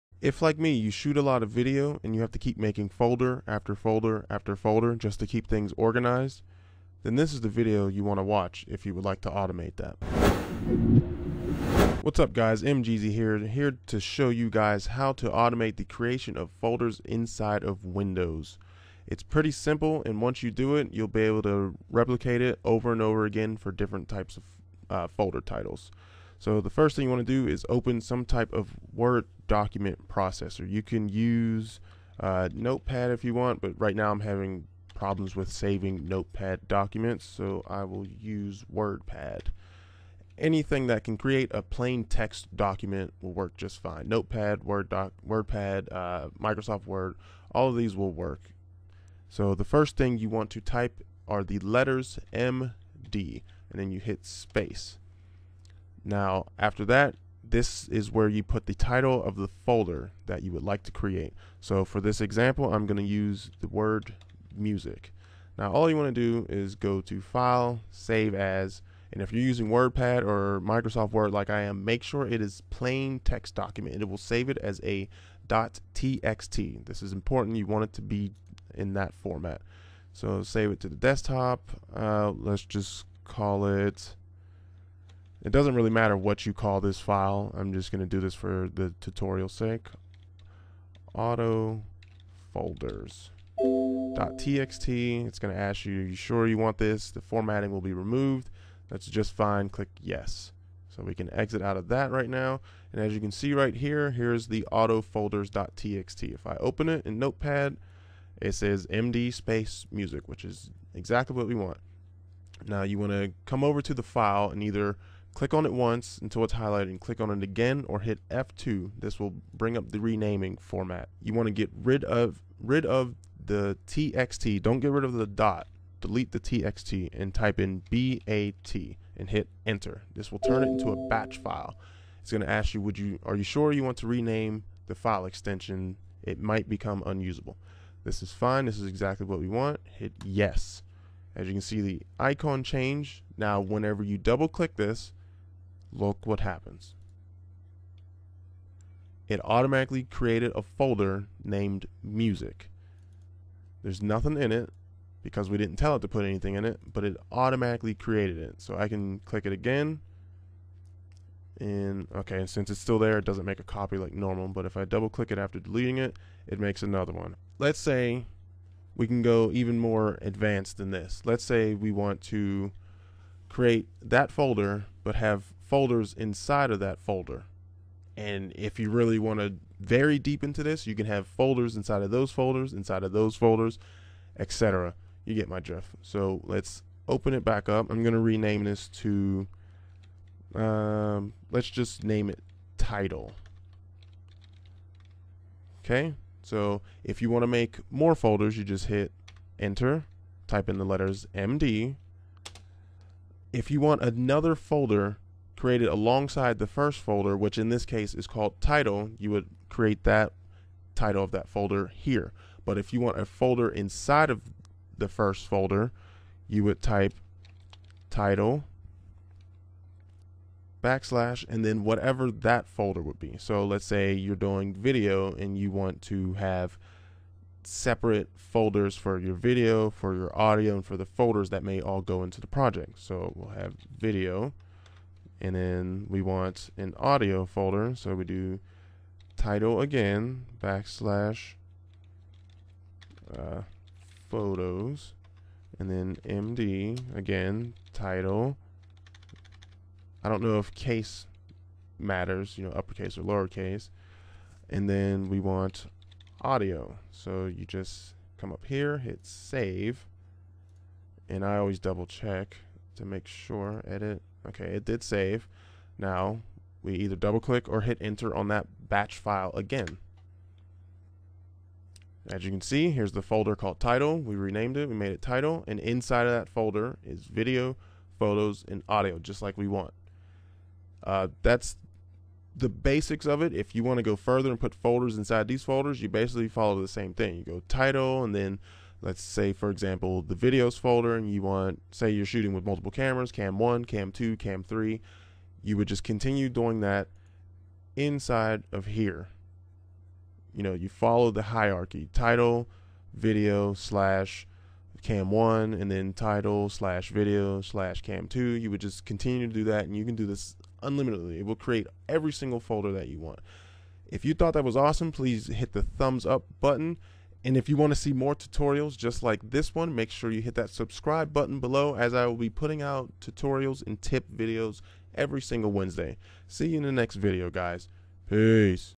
How To Automate Folder Creation
Do a lot of video or audio projects and hate creating countless folders like I do? Check this video out then.